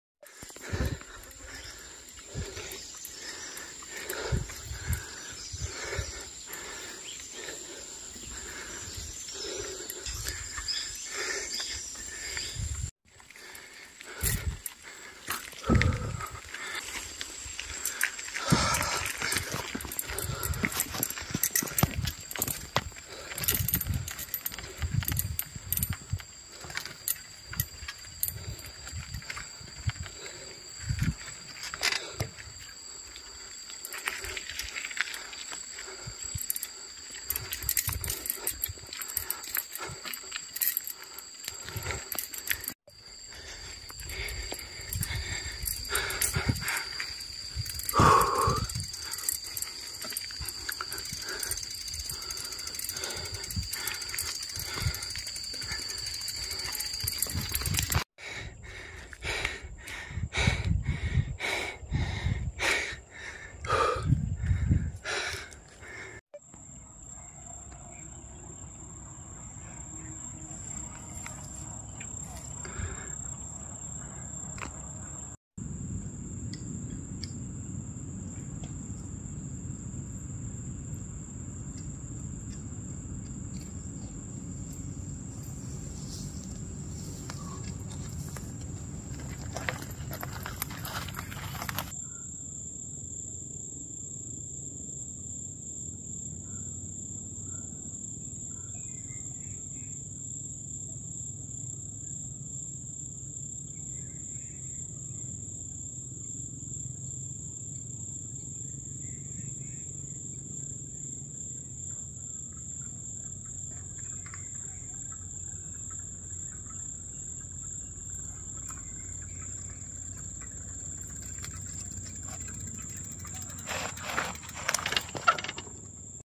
riding
FilmoraGo riding alone in mountain 🏔️🌄 mandi Himachal Pradesh